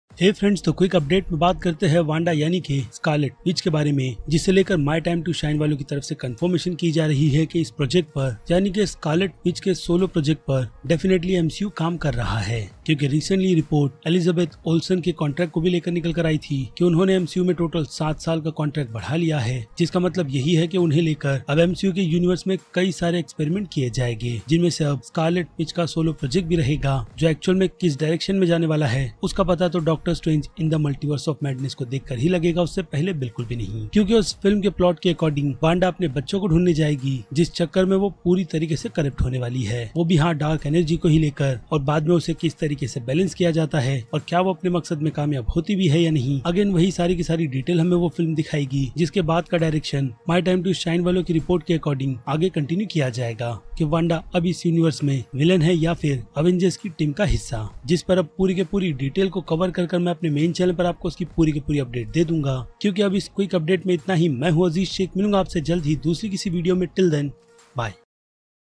Scarlet Witch Solo Project is in Work With MCU
Hello friend, today I am going to talk about scarlet witch solo project details in this video.